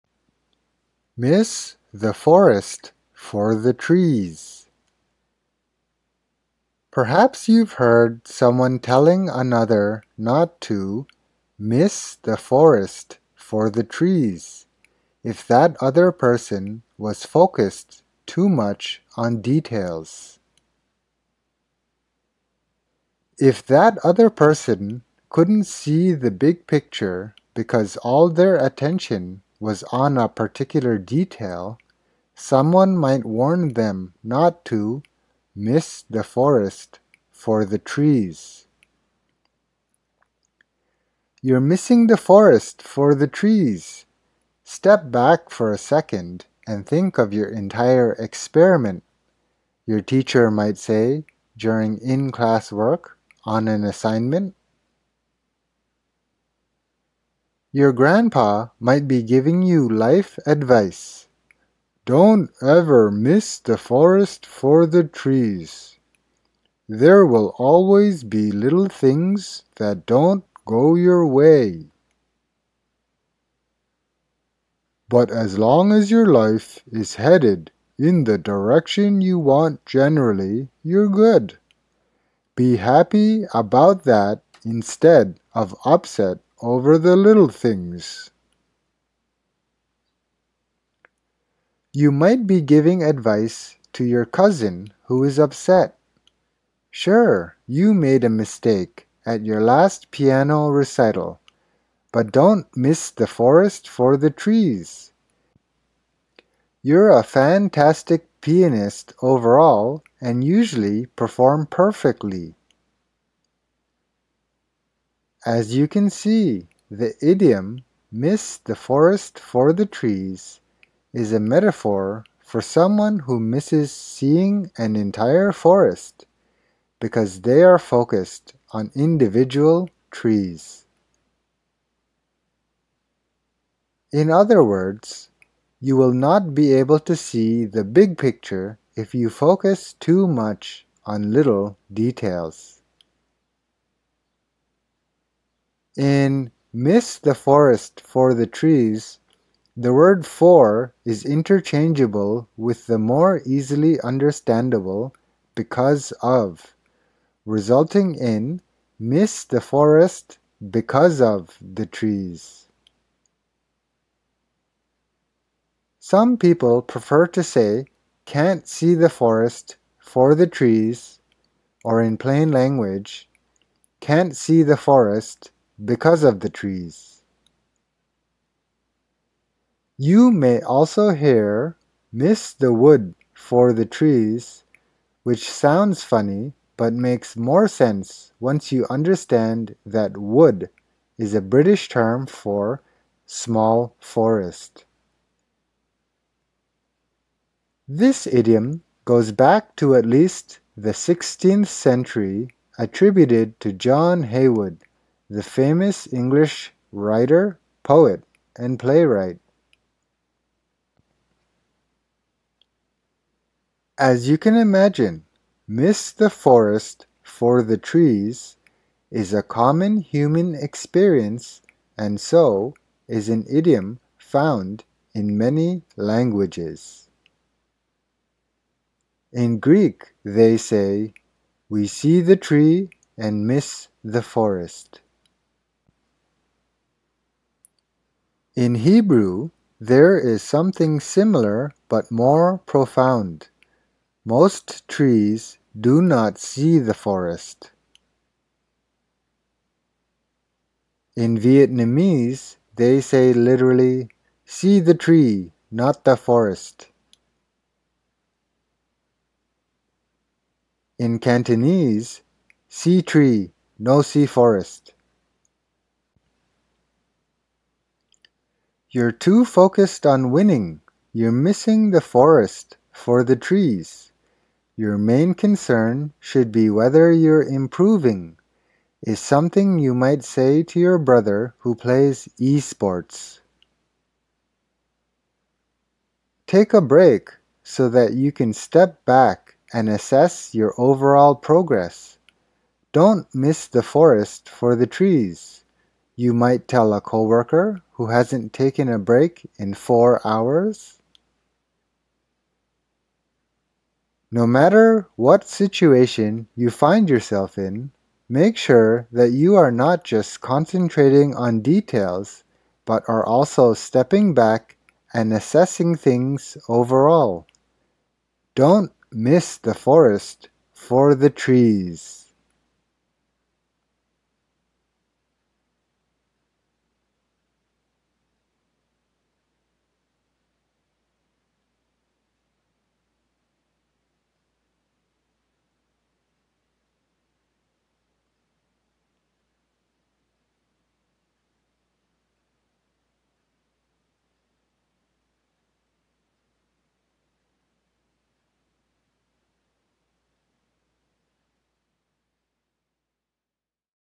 "Miss the Forest for the Trees" Idiom Meaning, Origin & History | Superduper English Idioms
0:00 - How to Say, “Miss the forest for the trees…”
0:40 - Examples of “Miss the forest for the trees…”
1:54 - Meaning of “Miss the forest for the trees…”
3:16 - Origin of “Miss the forest for the trees…”
4:27 - More Examples of “Miss the forest for the trees…”
5:03 - Conclusion

“Miss the forest for the trees…”

Perhaps you’ve heard someone telling another not to, “miss the forest for the trees,” if that other person was focused too much on details.

If that other person couldn’t see the big picture because all their attention was on a particular detail, someone might warn them not to, “miss the forest for the trees.”

“You’re missing the forest for the trees! Step back for a second and think of your entire experiment,” your teacher might say during in-class work on an assignment.

Your grandpa might be giving you life advice, “Don’t ever miss the forest for the trees; there will always be little things that don’t go your way…

… but as long as your life is headed in the direction you want generally, you’re good. Be happy about that instead of upset over the little things!”

You might be giving advice to your cousin who is upset, “Sure, you made a mistake at your last piano recital, but don’t miss the forest for the trees - you’re a fantastic pianist overall and usually perform perfectly.”

As you can see, the idiom, “miss the forest for the trees,” is a metaphor for someone who misses seeing an entire forest, because they are focused on individual trees.

In other words, you will not be able to see the big picture if you focus too much on little details.

In “miss the forest for the trees”, the word “for” is interchangeable with the more easily understandable, “because of”, resulting in “miss the forest because of the trees”.

Some people prefer to say, “can’t see the forest for the trees”, or in plain language, “can’t see the forest because of the trees”.

You may also hear, “miss the wood for the trees,” which sounds funny, but makes more sense once you understand that “wood” is a British term for “small forest”.

This idiom goes back to at least the 16th century, attributed to John Heywood, the famous English writer, poet and playwright.

As you can imagine, “miss the forest for the trees,” is a common human experience and so is an idiom found in many languages.

In Greek, they say, “we see the tree and miss the forest.”

In Hebrew, there is something similar but more profound, “most trees do not see the forest.”

In Vietnamese, they say literally, “see the tree, not the forest.”

In Cantonese, “see tree, no see forest.”

“You’re too focused on winning, you’re missing the forest for the trees. Your main concern should be whether you’re improving,” is something you might say to your brother who plays esports.

“Take a break so that you can step back and assess your overall progress, don’t miss the forest for the trees,” you might tell a co-worker who hasn’t taken a break in four hours.

No matter what situation you find yourself in, make sure that you are not just concentrating on details but are also stepping back and assessing things overall. Don’t miss the forest for the trees!